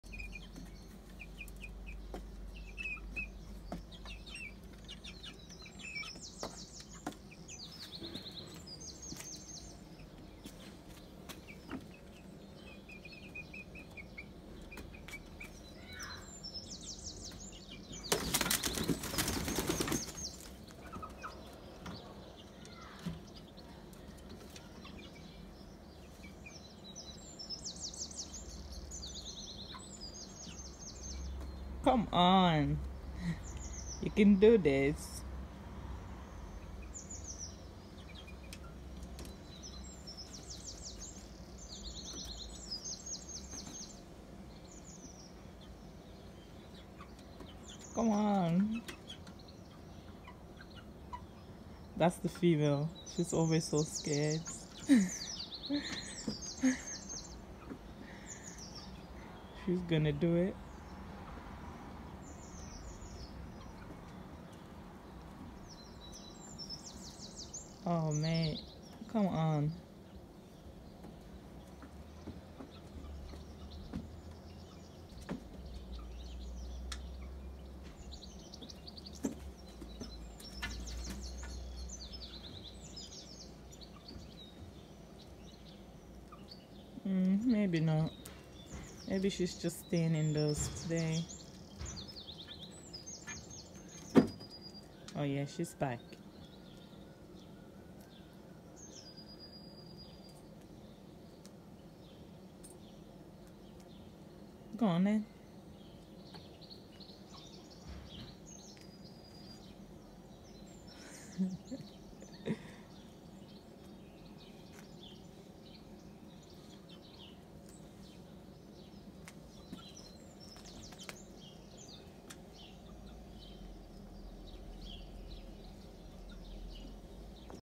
Growing chicken in our backyard for eggs, living in a city and want to get some organic eggs?
We have started growing our own chicken in our backyard mainly to get fresh eggs but you live in the city?
 This is just a little DIY Coop we made ourselves. We live in the city and thought this will be very difficult but it actually isn't and most of all it's so fun and rewarding.

You should try it too if you have been thinking about it.